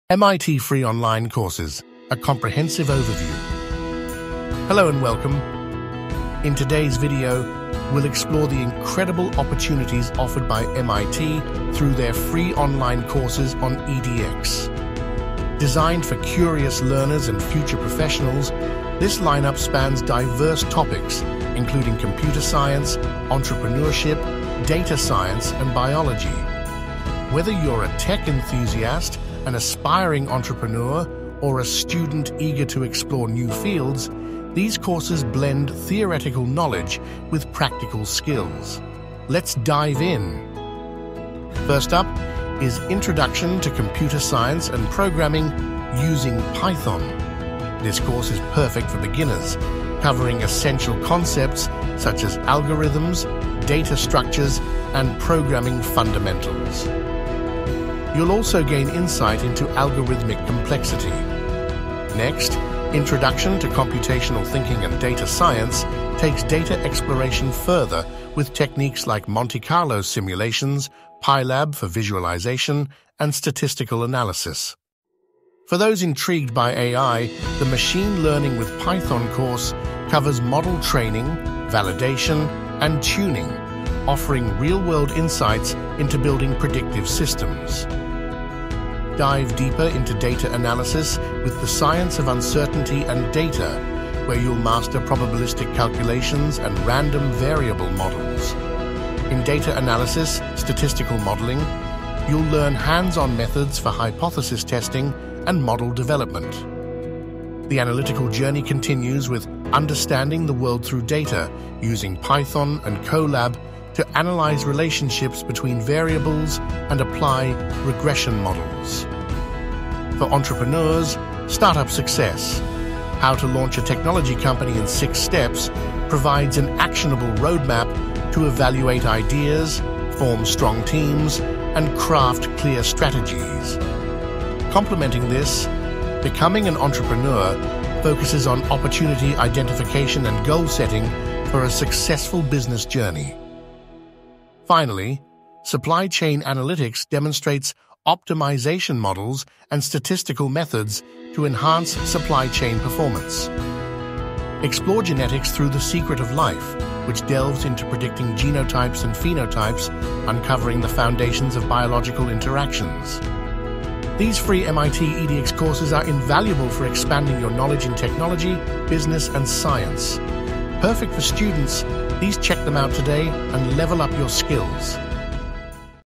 I Took MIT's Free Online Courses (And They're Amazing)
In this video, I share my incredible experience taking MIT's free online courses, and I'm blown away by the quality of education I received. From the comfort of my own home, I was able to access top-notch lectures and learn from renowned professors at one of the world's most prestigious institutions. With MIT's free online courses, you can gain valuable knowledge and skills without breaking the bank. Whether you're a student looking to supplement your education or a professional seeking to upskill, MIT's online courses are an amazing resource that can help you achieve your goals. So, if you're curious about online learning and want to know more about MIT's free courses, watch this video to find out my honest review and what you can expect from this incredible opportunity.